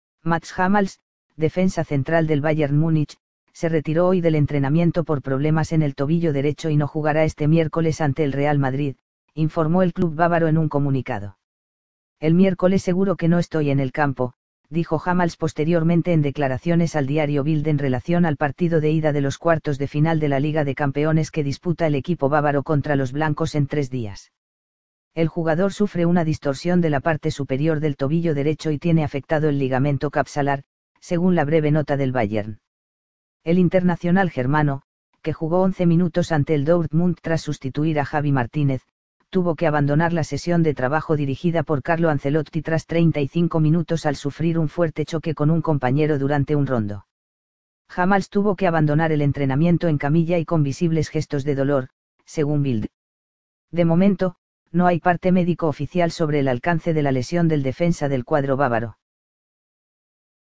Bayern pierde a Hummels para enfrentar al Real Madrid!
.Matts Hummels, defensa central del Bayern Múnich, se retiró hoy del entrenamiento por problemas en el tobillo derecho y no jugará este miércoles ante el Real Madrid, informó el club bávaro en un comunicado.

“El miércoles seguro que no estoy en el campo”, dijo Hummels posteriormente en declaraciones al diario Bild en relación al partido de ida de los cuartos de final de la Liga de Campeones que disputa el equipo bávaro contra los blancos en tres días.

El jugador sufre una “distorsión de la parte superior del tobillo derecho” y tiene afectado el “ligamento capsular”, según la breve nota del Bayern.

El internacional germano, que jugó 11 minutos ante el Dortmund tras sustituir a Javi Martínez, tuvo que abandonar la sesión de trabajo dirigida por Carlo Ancelotti tras 35 minutos al sufrir un fuerte choque con un compañero durante un rondo.

Hummels tuvo que abandonar el entrenamiento en camilla y con visibles gestos de dolor, según Bild.

De momento, no hay parte médico oficial sobre el alcance de la lesión del defensa del cuadro bávaro.